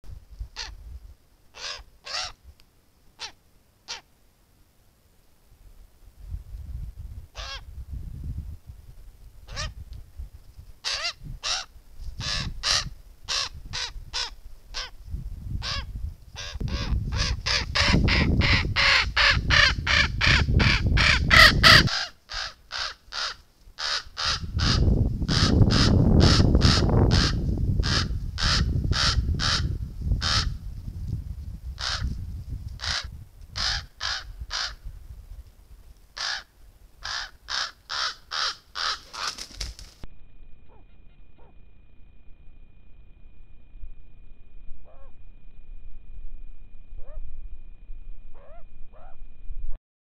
Очень красивый и важный ворон.
Этот ворон был снят на фотоловушку в Мурманских сопках.